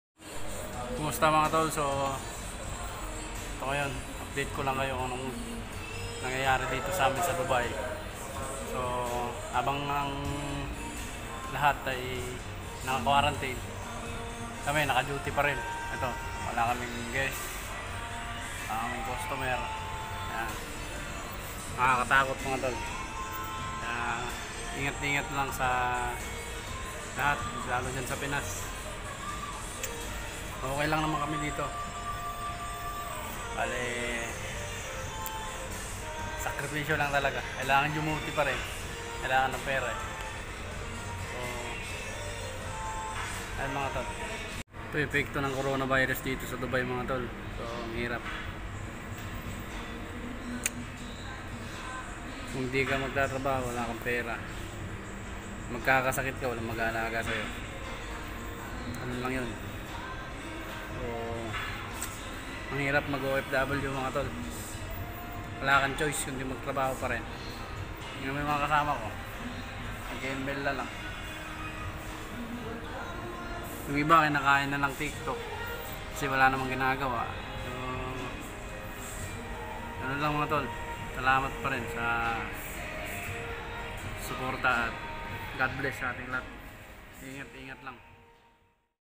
CORONA VIRUS(COVID-19) EFFECT TO ALL BUSINESS AND ALL COMPANY IN DUBAI
Update ko lng kayo mga ka tropa sa sitwasyon namin dto sa Dubai gawa ng Corona Virus na yan. Ingat lagi sa ating lahat saan man sulok ng mundo...